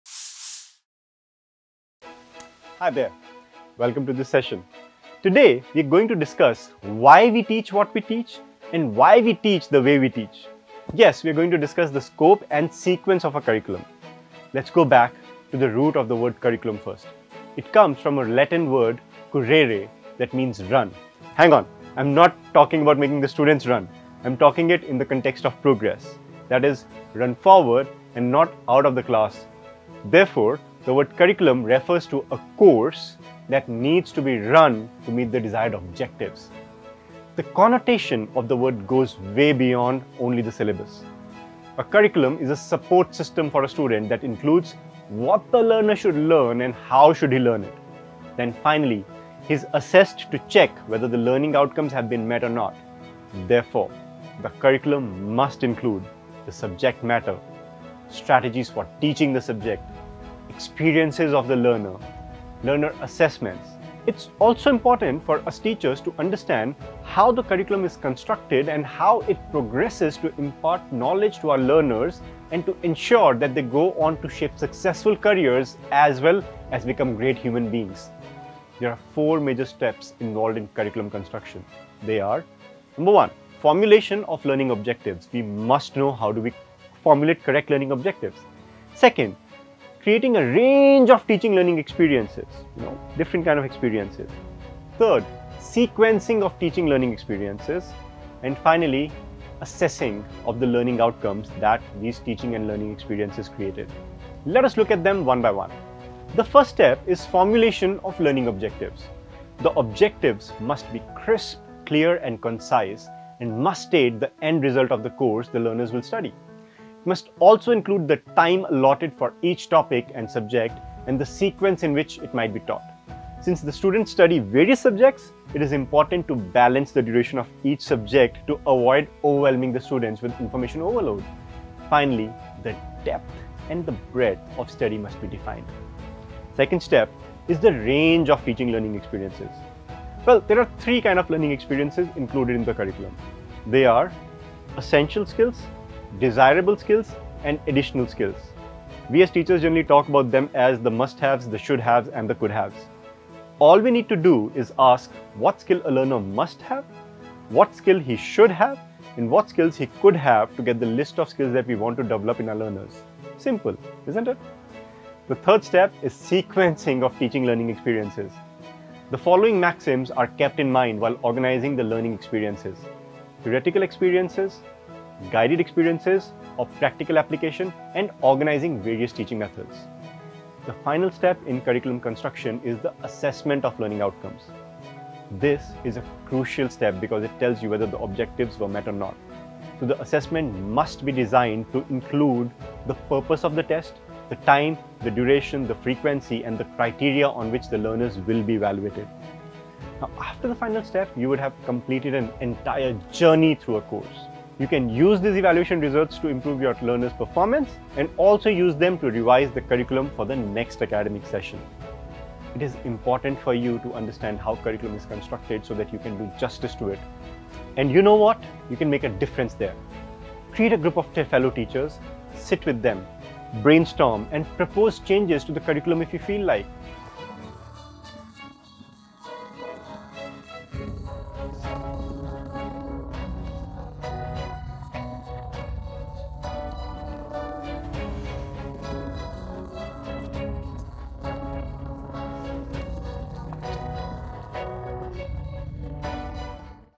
Does curriculum mean syllabus? | Tips for Curriculum construction | Suraasa
Are the curriculum and syllabus different? 🤔

In this session, we delve into the essential differences between the two and explore the intricacies of curriculum construction. 📚✏️ Learn why we teach what we teach and how to design a curriculum that goes beyond just listing topics. From understanding the origin of the word "curriculum" to mastering the four key steps of curriculum construction—formulating learning objectives, creating diverse teaching experiences, sequencing those experiences, and assessing learning outcomes—this video offers valuable insights for educators. 🎓

Join us as we break down each step to help you build a curriculum that not only meets educational goals but also shapes successful careers and well-rounded individuals. 🌟 Whether you're a seasoned educator or just starting out, these tips will help you make a meaningful impact in your teaching journey. 💡🚀

👉Ready to connect with a Suraasa Mentor for guidance on navigating your teaching career?